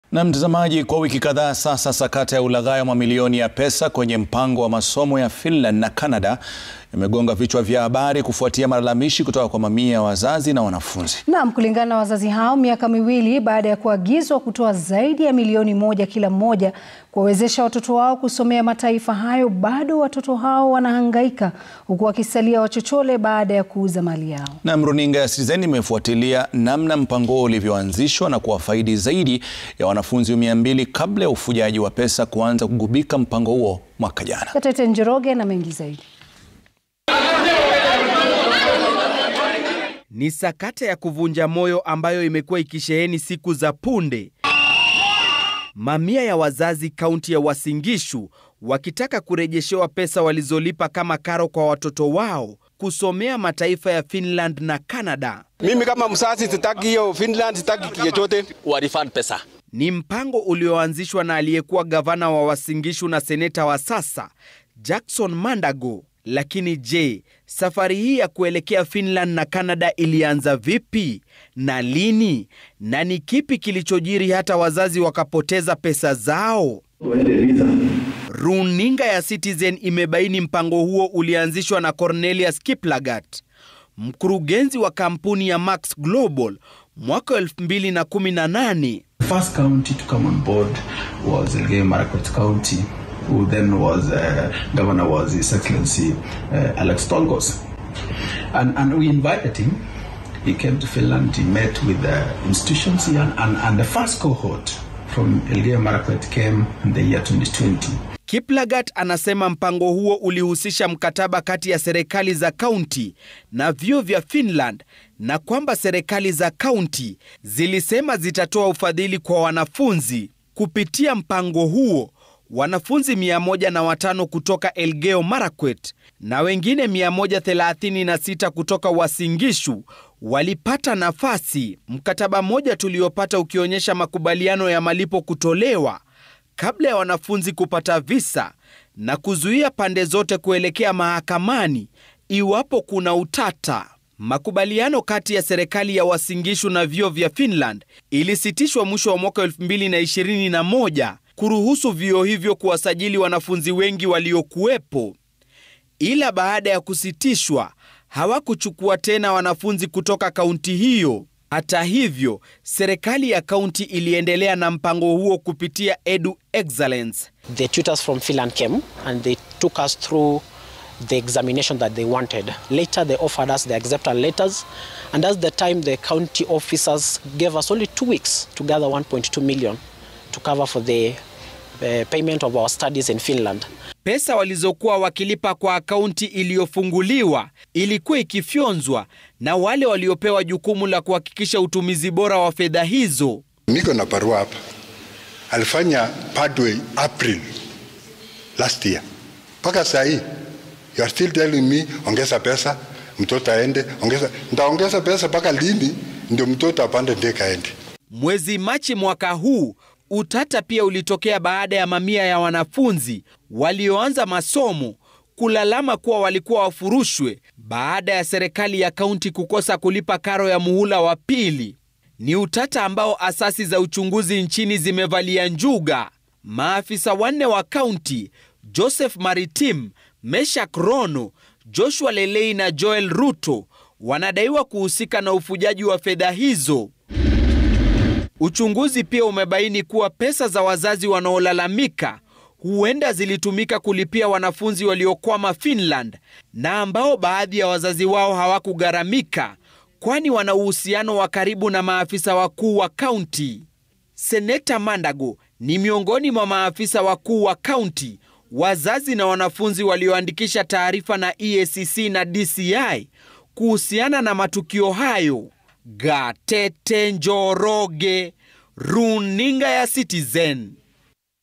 Sakata ya elimu ya Finland
Kwa wiki kadhaa sasa, sakata ya ulaghai wa mamilioni ya pesa kwenye mpango wa masomo ya Finland na Canada imegongwa vichwa vya habari kufuatia malalamishi kutoka kwa mamia ya wazazi na wanafunzi.  Kulingana na wazazi hao miaka miwili baada ya kuagizwa kutoa zaidi ya milioni moja kila mmoja kuwawezesha watoto wao kusomea mataifa hayo bado watoto wao wanahangaika huku wakisalia wachochole baada ya kuuza mali yao. Runinga ya Citizen imefuatilia namna mpango huo ulivyoanzishwa na kuwafaidi zaidi ya wanafunzi 200 kabla ya ufujaji wa pesa kuanza kugubika mpango huu mwaka jana.